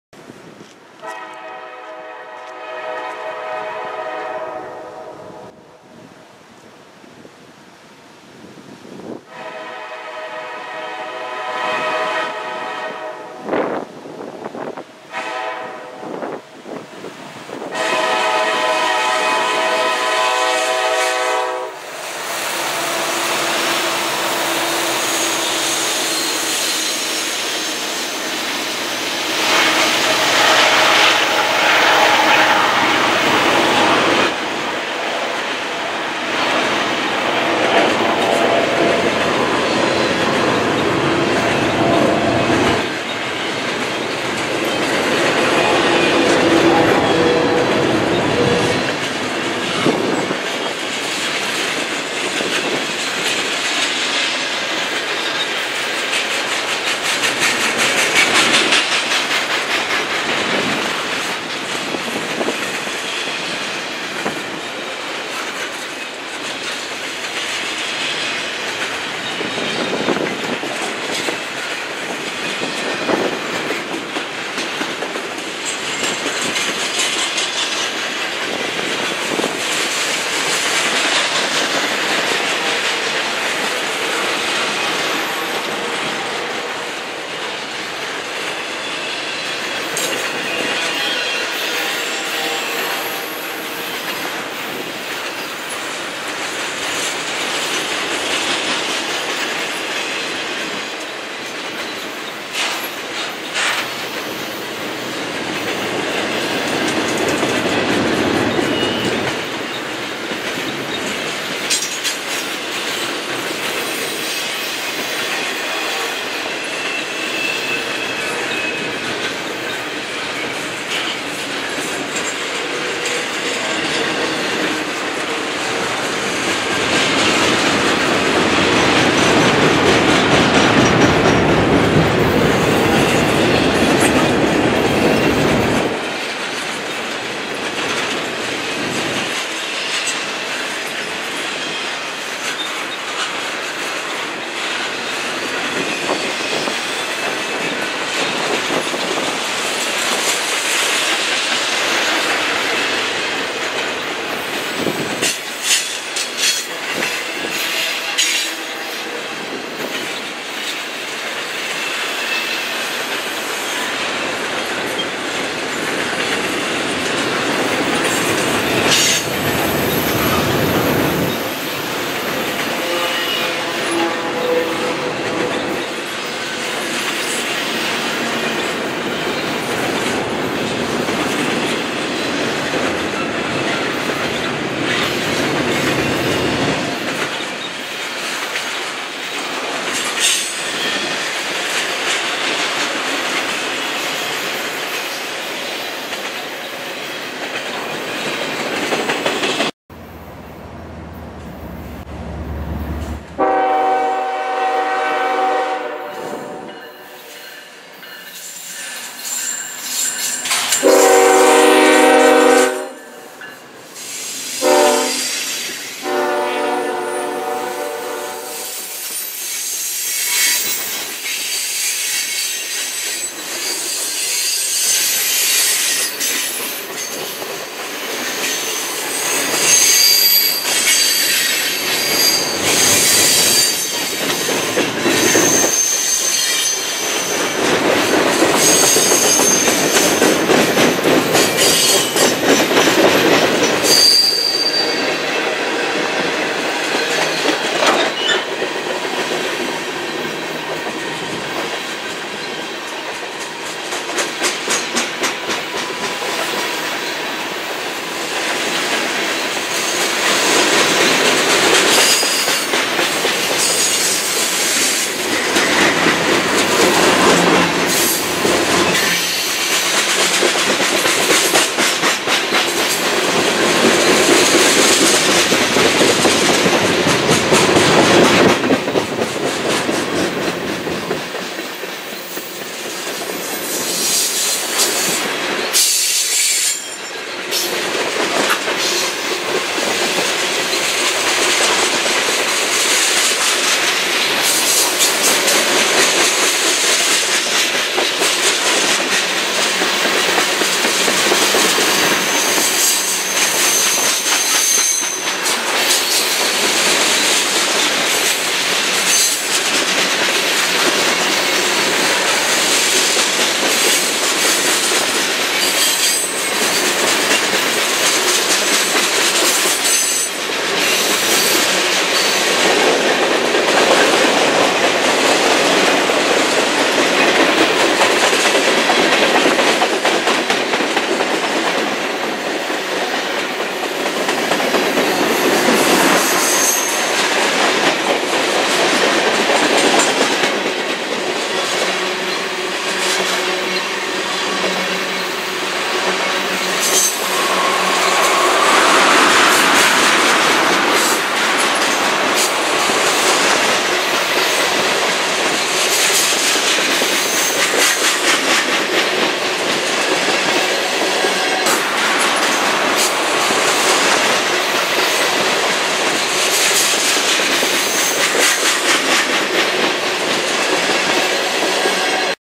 1 Intermodal and 1 Mixed freight with Tags Norfolk Southern Fort Wayne Line
Here are two run bys 1 Intermodal and 1 Mixed freight with Tags.